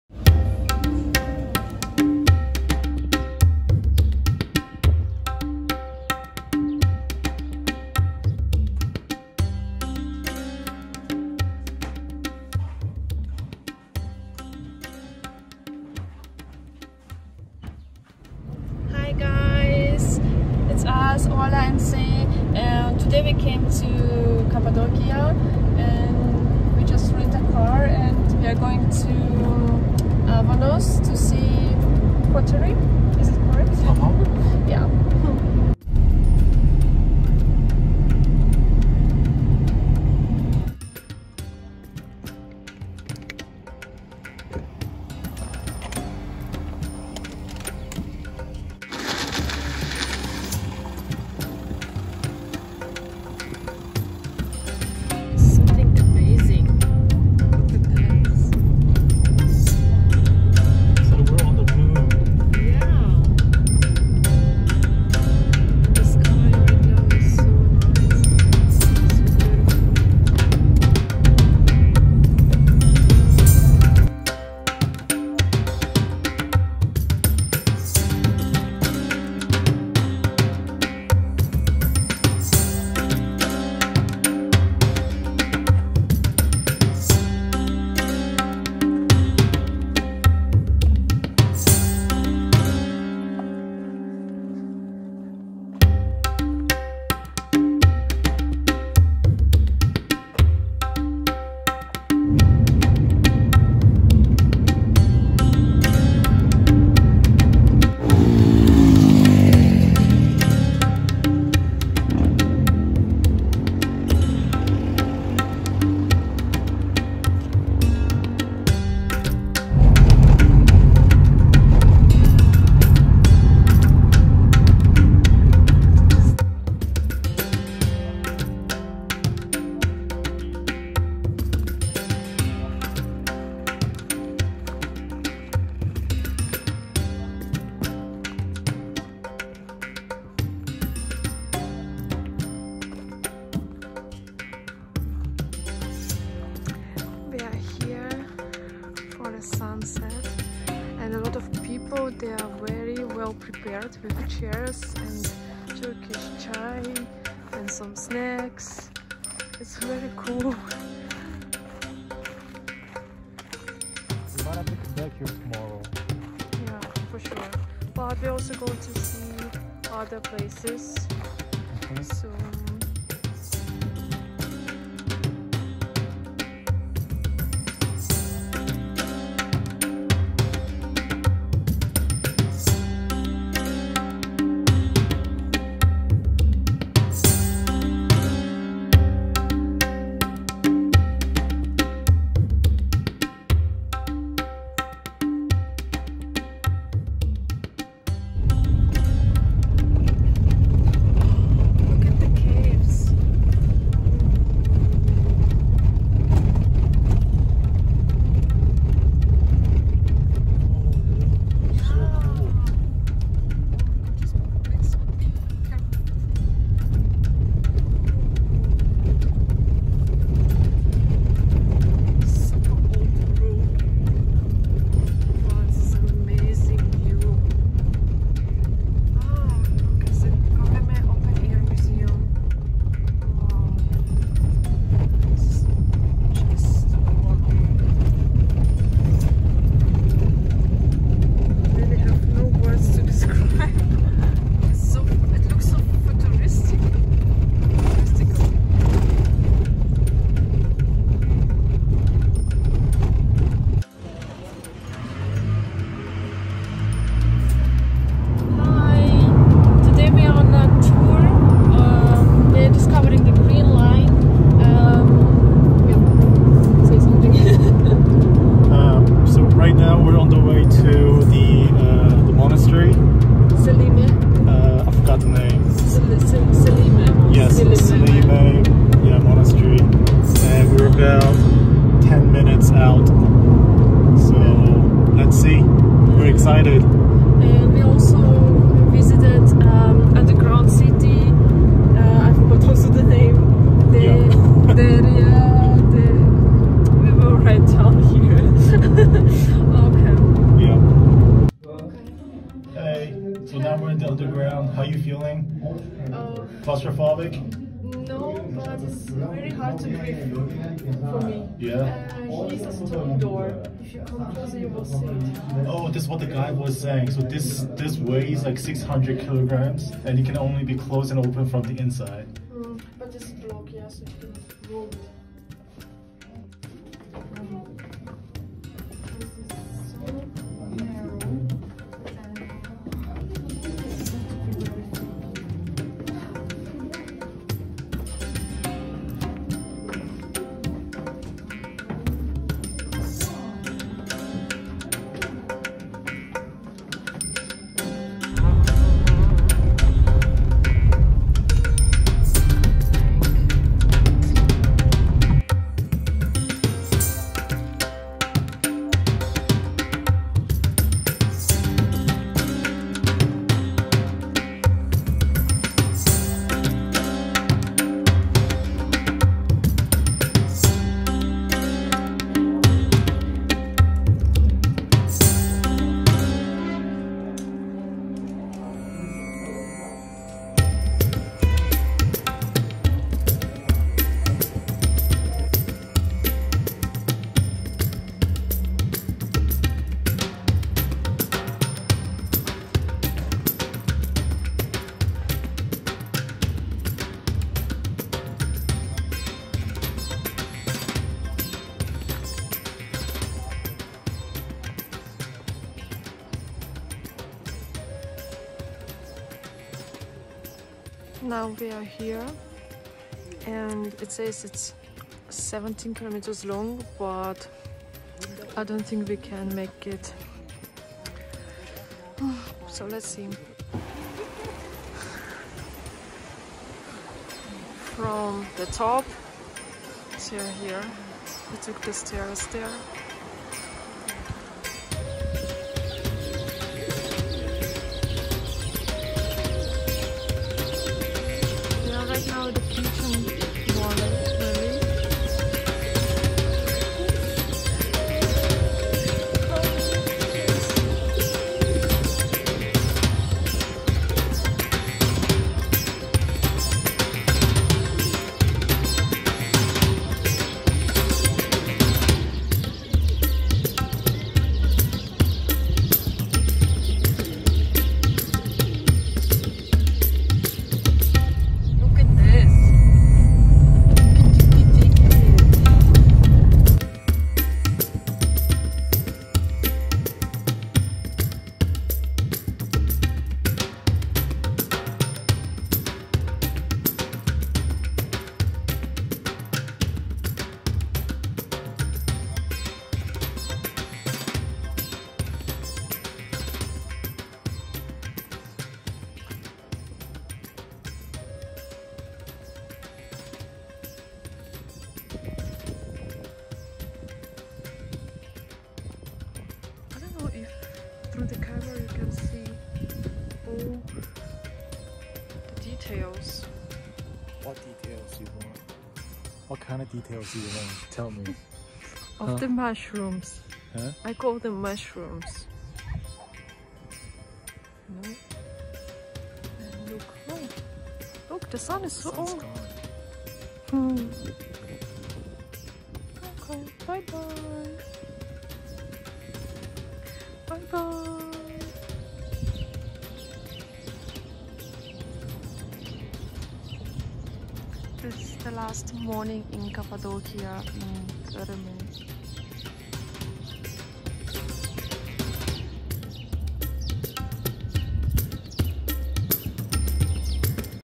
Cappadocia vlog
We cramped Cappadocia in our short trip to Turkey. It was tiring but was absolutely worth it!